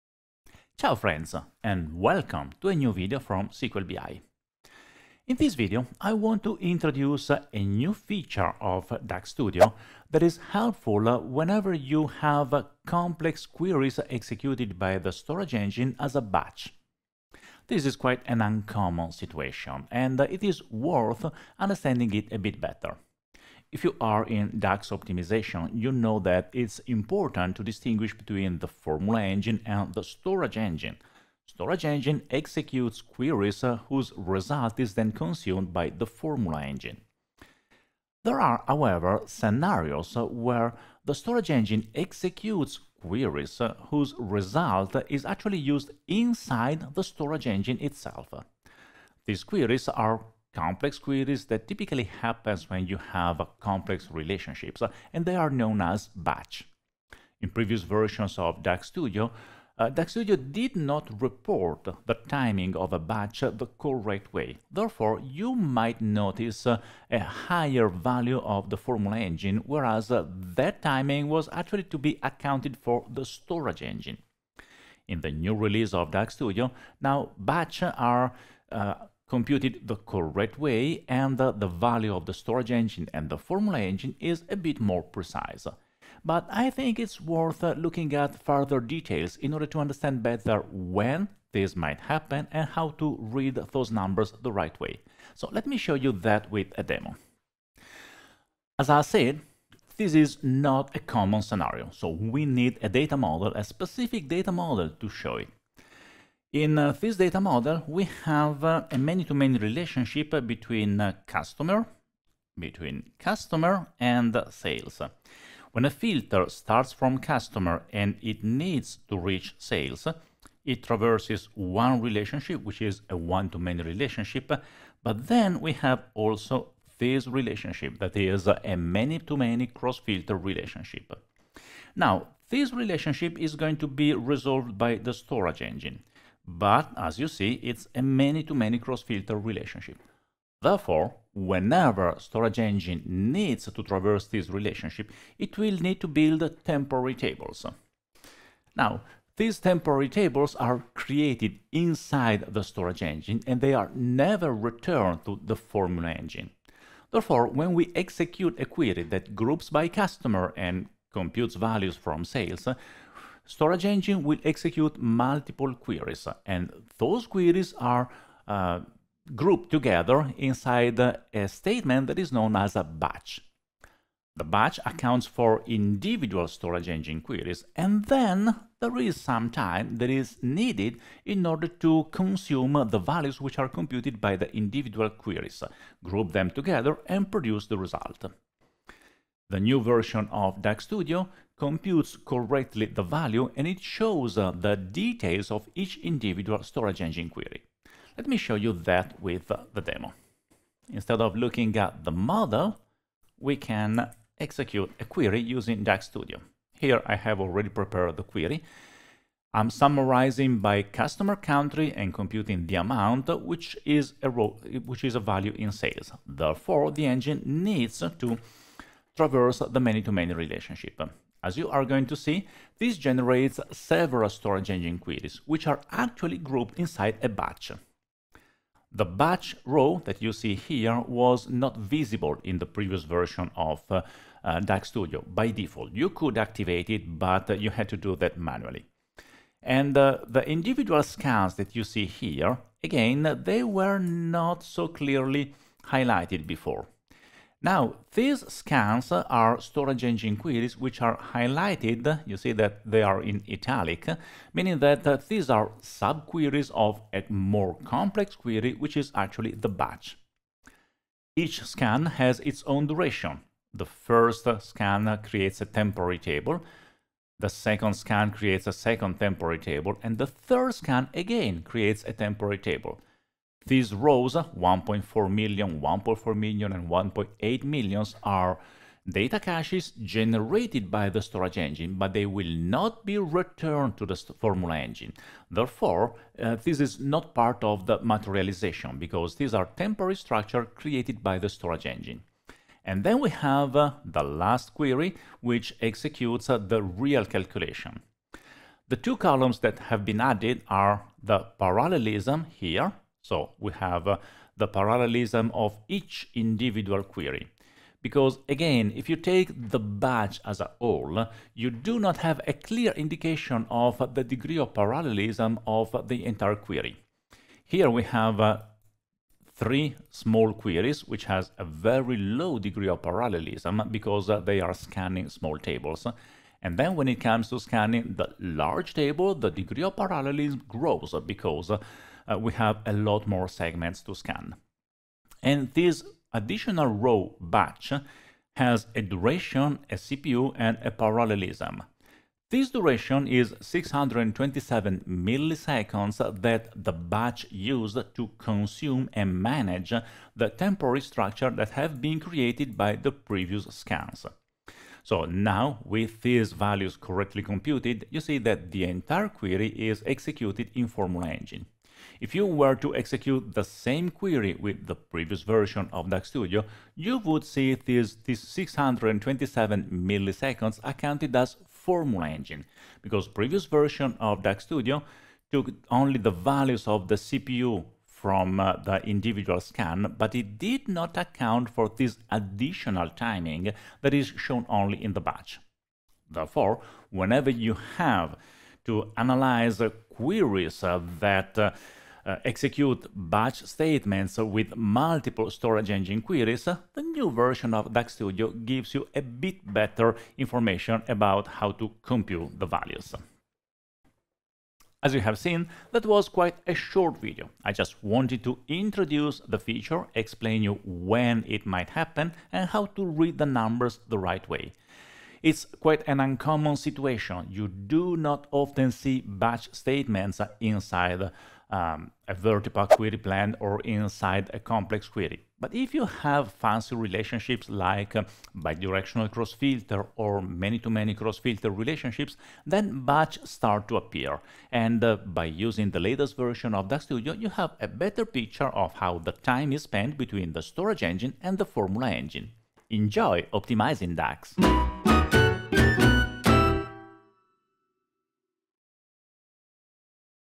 Analyzing the parallelism of storage engine operations in DAX Studio
DAX Studio 2.17 introduced improved server timings features to analyze the parallelism of storage engine operations.